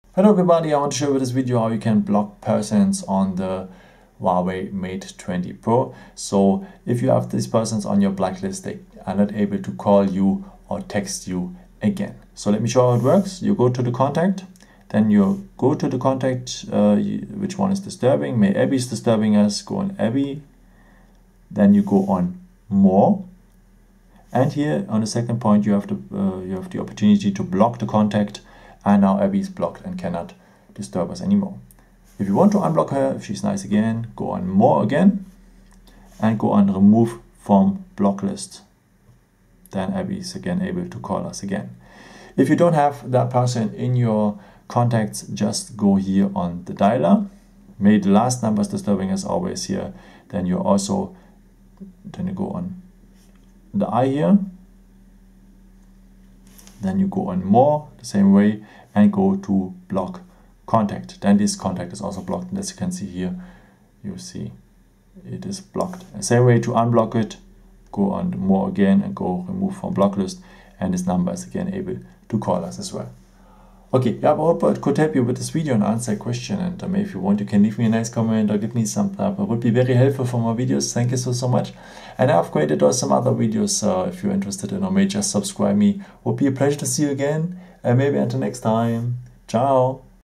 Huawei Mate 20 (Pro): How to block contacts block calls & texts from disturbing persons (Blacklist)
With this video, I want to show you, how you can block persons in your contact and dialer list and how you can unblock the number again at the Huawei Mate 20 and Mate 20 Pro.
All the blocked contacts on your blocklist cannot reach you and you are not receiving their text messages.
I have used a Huawei Mate 20 Pro for that video.